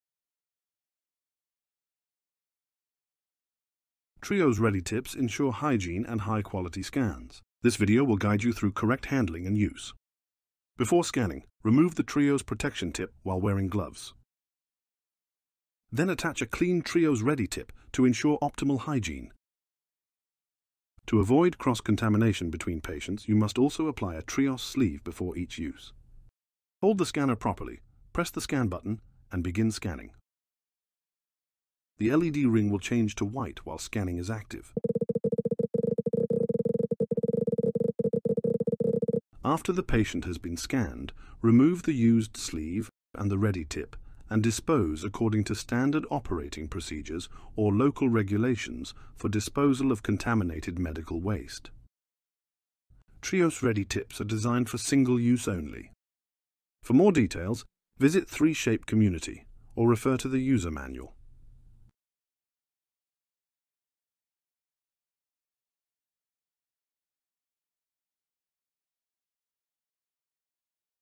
Working with the TRIOS Ready Tip.
The Ready Tip helps you maintain the proper positioning for optimal intraoral access . In this video, you will learn practical techniques for using this feature effectively in your daily workflow.
Save the video to access it anytime and share it with colleagues who use the TRIOS 6. We'll be back next week with another helpful tip to make your scanning easier.
Watch now to learn how the Ready Tip works with your TRIOS 6.
TRIOS Ready Tip is not sterilized and not appropriate for all clinical situations.